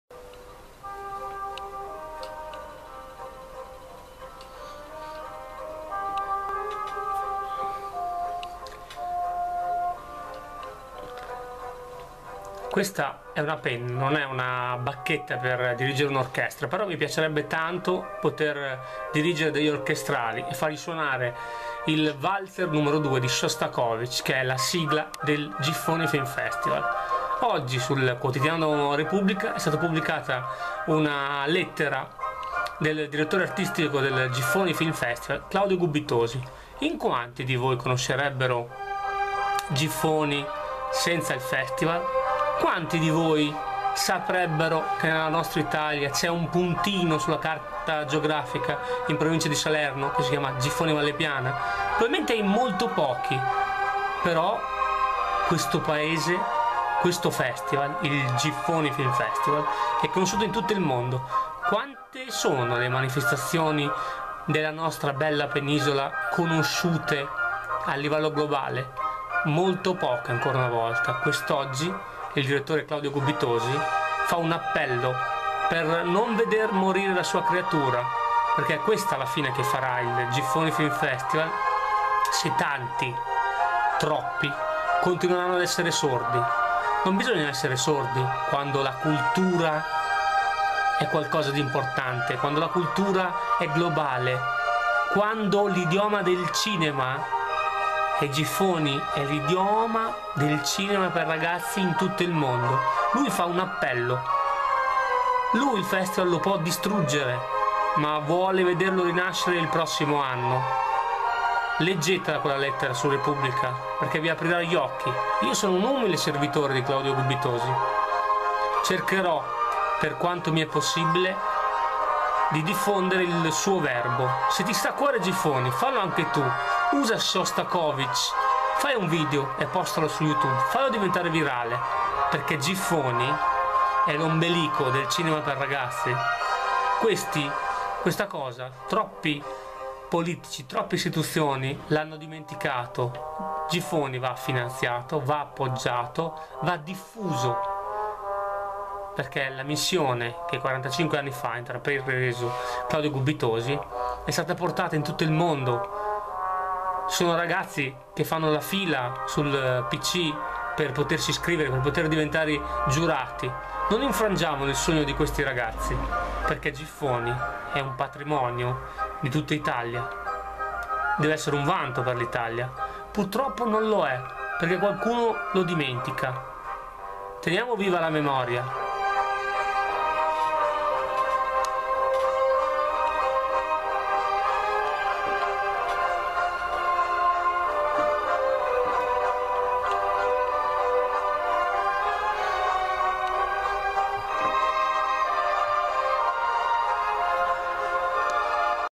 IOSONOGIFFONI#
Sul quotidiano La Repubblica il direttore artistico del Giffoni Film Festival Claudio Gubitosi lancia il suo grido d'allarme.